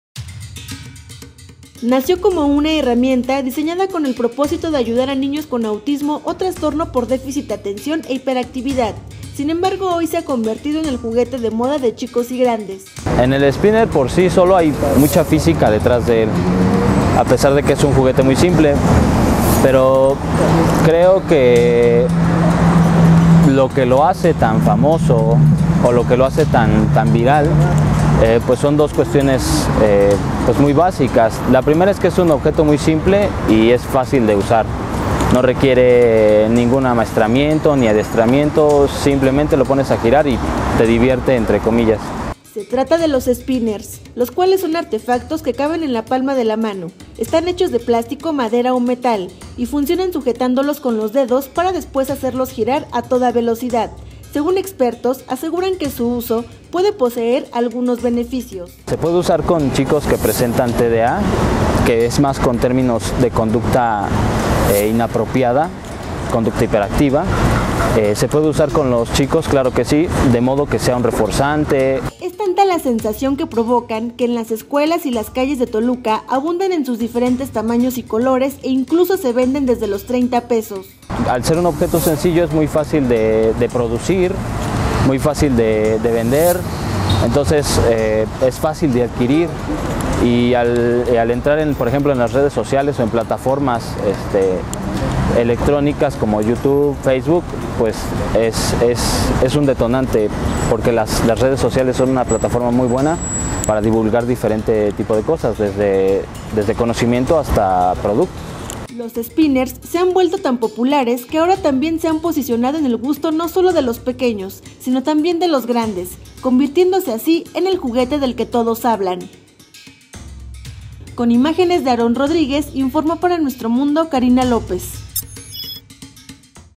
Spinner, juguete de moda de chicos y grandes
Nació como una herramienta diseñada con el propósito de ayudar a niños con autismo o trastorno por déficit de atención e hiperactividad; sin embargo hoy se ha convertido en el juguete de moda de chicos y grandes.

“El spinner cuenta con mucha física, a pesar de que es un juguete muy simple, pero creo que lo que lo hace tan famoso son dos cuestiones básicas, ya que es fácil de usar, no requiere ningún amaestramiento ni adiestramiento, simplemente lo pones a girar y te divierte entre comillas”, indicó el psicólogo Daniel Cabrera.